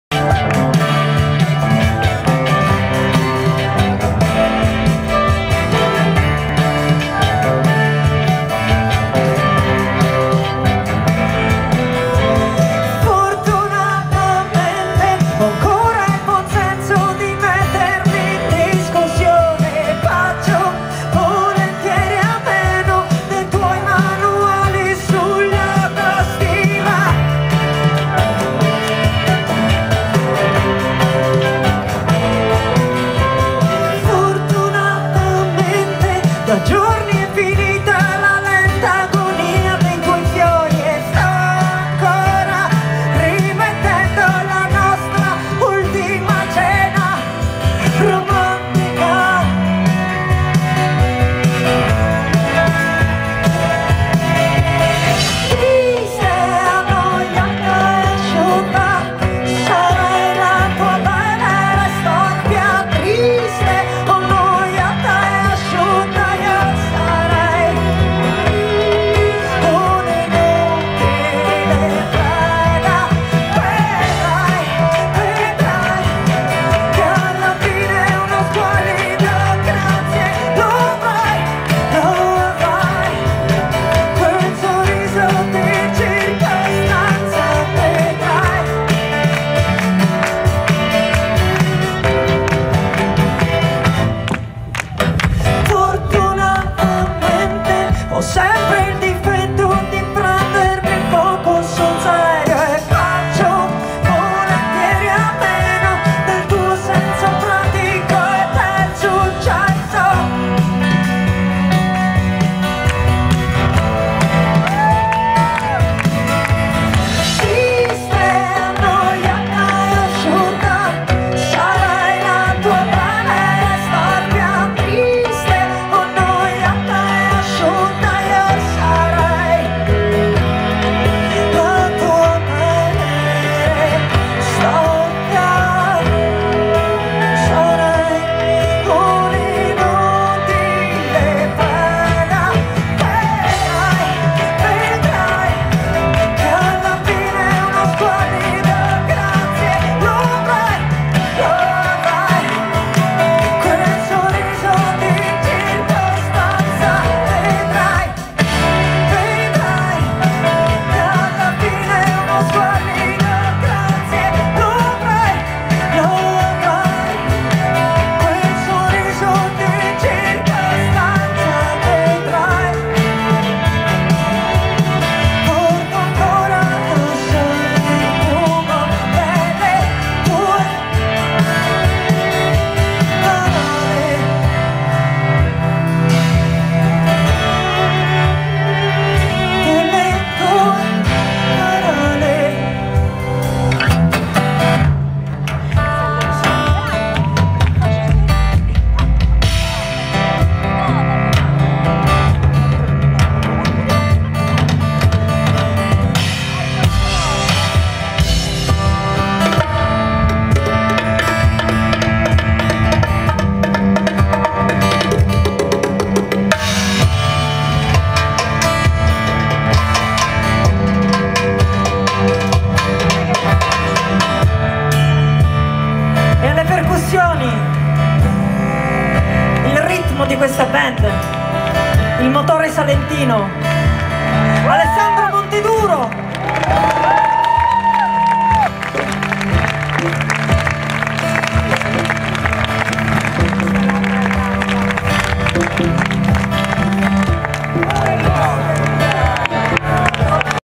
Carmen Consoli in concerto -
Estratto video dal concerto del 13 agosto 2017 tenuto dalla Consoli nella cittadina di Ostuni in provincia di Brindisi.